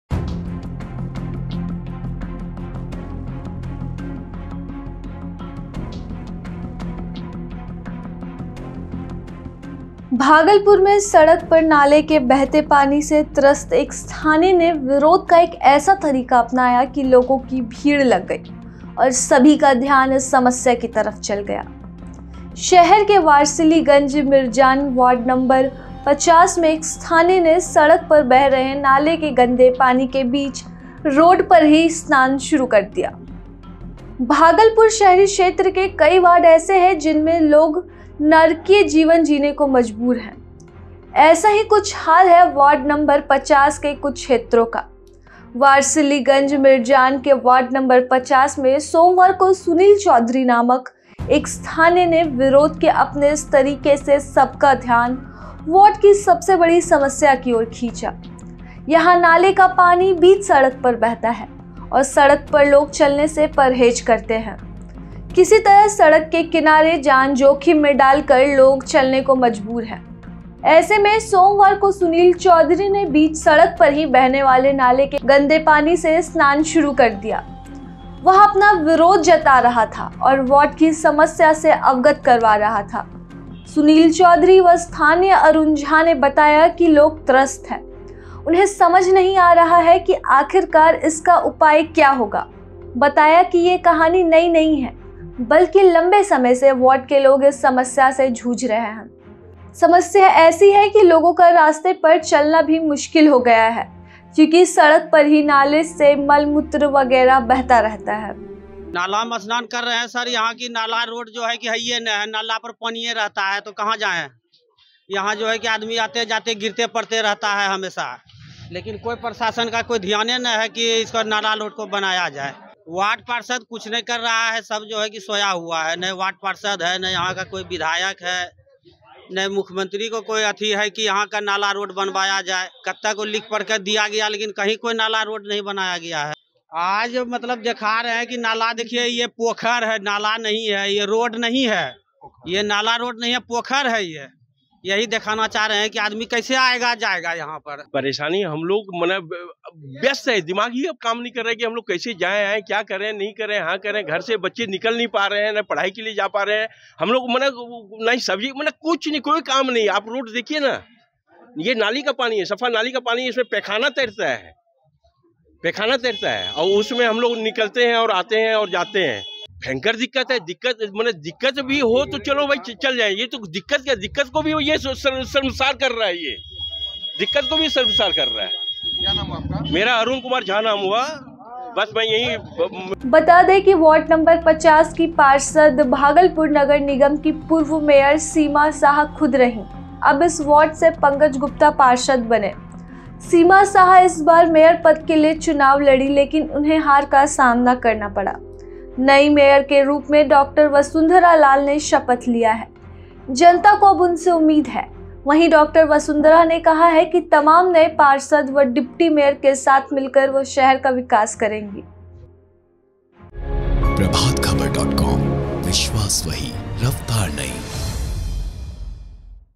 Bhagalpur में बीच सड़क पर बह रहे नाले के गंदे पानी से स्नान, त्रस्त स्थानीय ने ऐसे जताया विरोध
भागलपुर के वार्ड नंबर 50 में बीच सड़क पर बह रहे नाले के गंदे पानी से स्नान करके स्थानीय एक नागरिक ने विरोध जताया. देखिये वीडियो.. | Bihar News: भागलपुर में सड़क पर नाले के बहते पानी से त्रस्त एक स्थानीय ने विरोध का ऐसा तरीका अपनाया कि लोगों की भीड़ लग गयी और सभी का ध्यान इस समस्या की तरफ चला गया. शहर के वारसिलीगंज मिरजान के वार्ड नंबर 50 में एक स्थानीय ने सड़क पर बह रहे नाला के पानी से बीच रोड पर स्नान करना शुरू कर दिया.